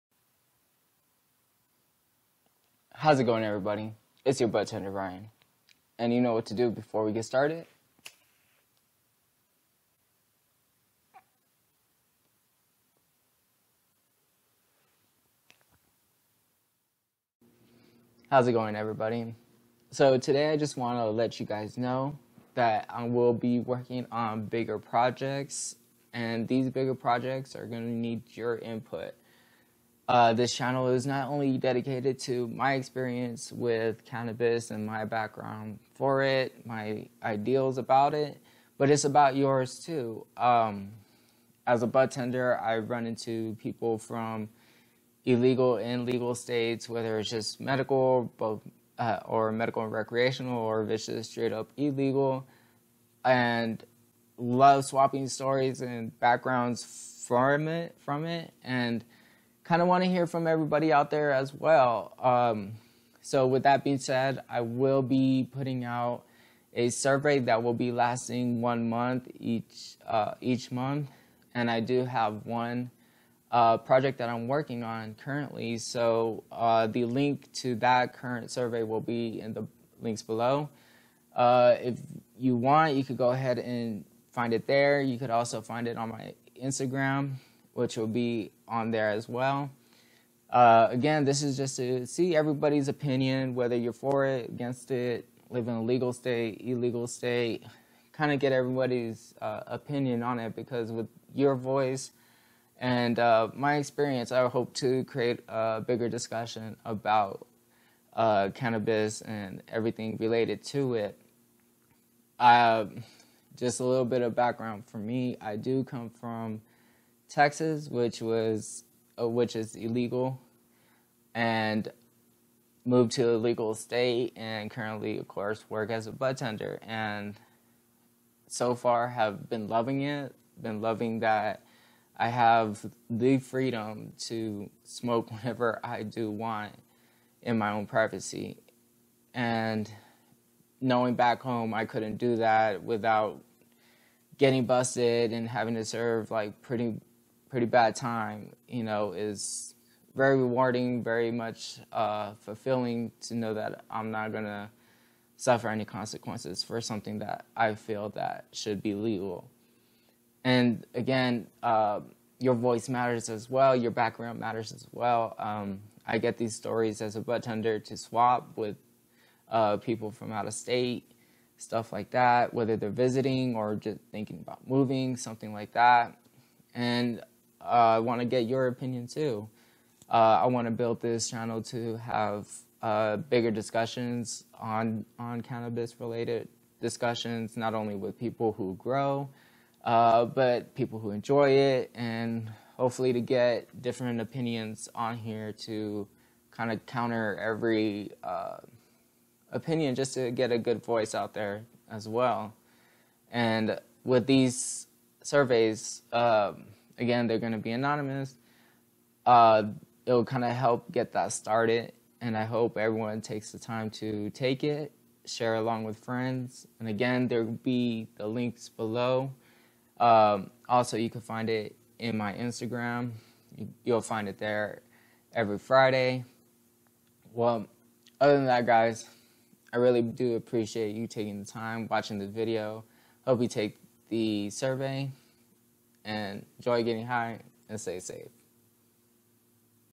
Your Voice Matters! Bigger Discussions Coming!
I believe with everyones input on these anonymous surveys we can have larger discussions on cannabis.
Even as a budtender I love to hear every guests experience with cannabis, because we all have our stories and ideas about it and it's great to hear the different ways we all enjoy cannabis.